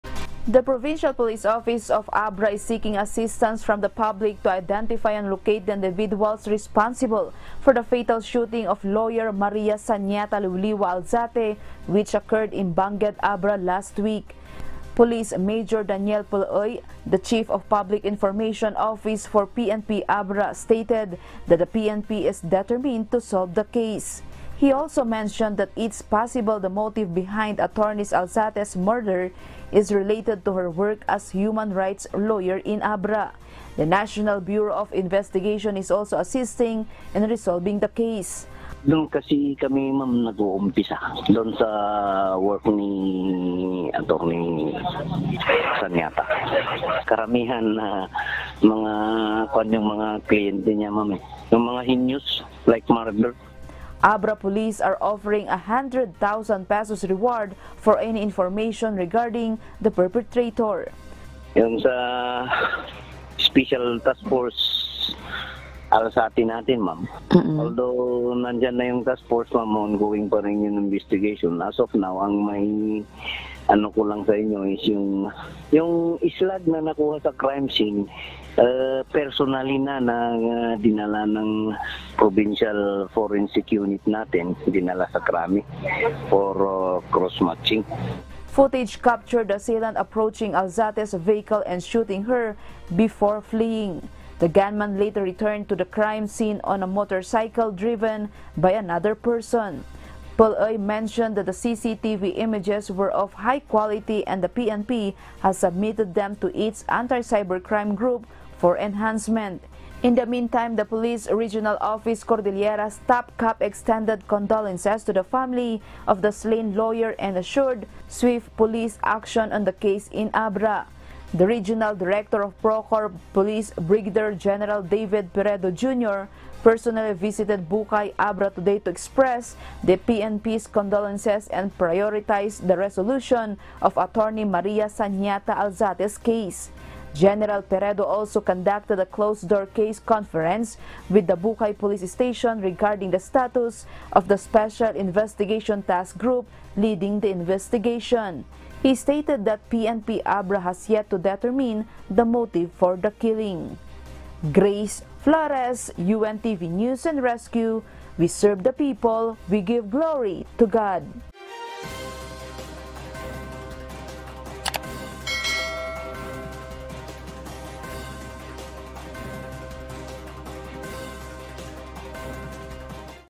Abra PNP sets up P100K bounty for capture of Bangued lawyer slay suspects
The Philippine National Police in Abra offers P100,000 reward for any information leading to the apprehension of the suspects responsible for the killing of a lawyer in Abra province.